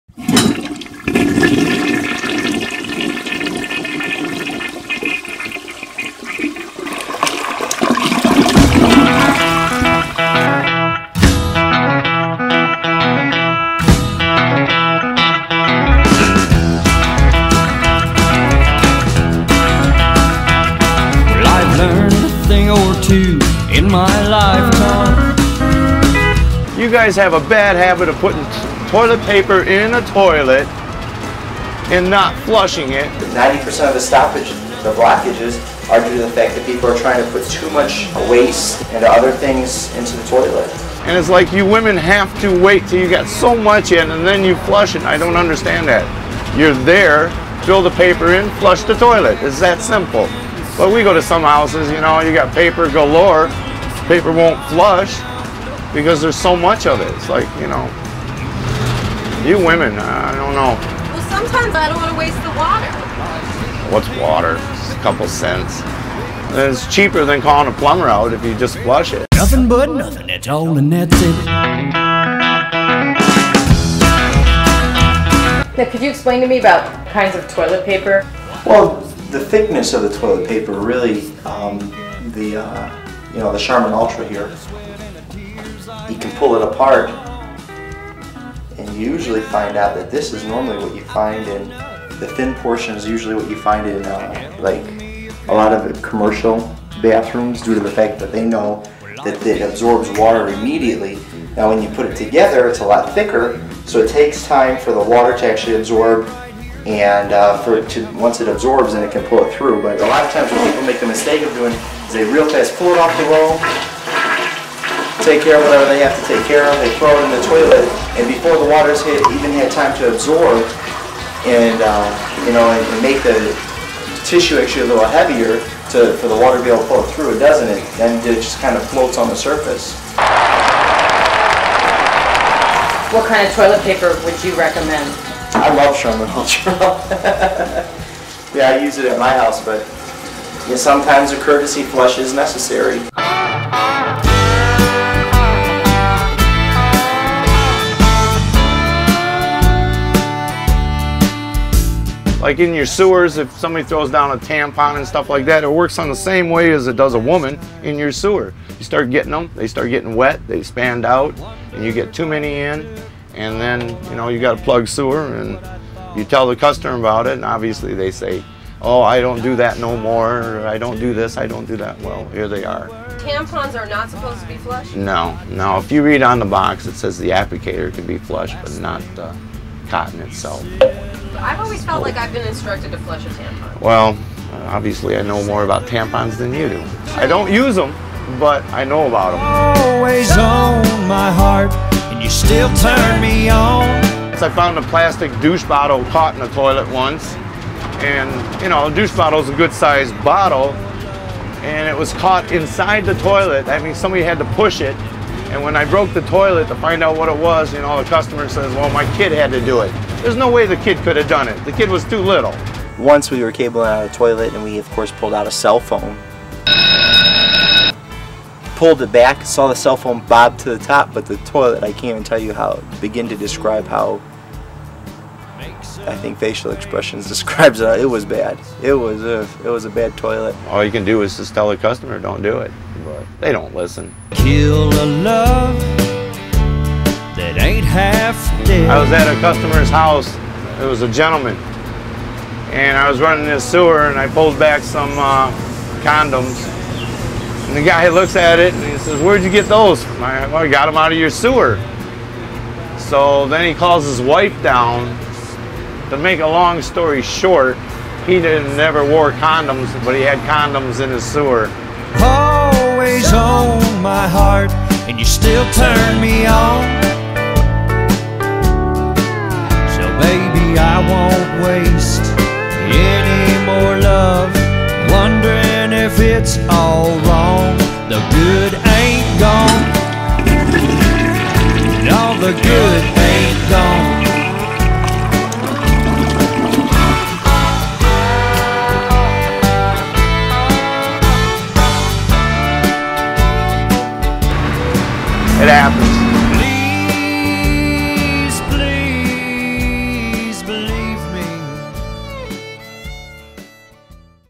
FLUSH TV #4: What Not to Flush (Five Lessons to Live By)
Everyone knows you can't flush a cell phone down the toilet ... or do they?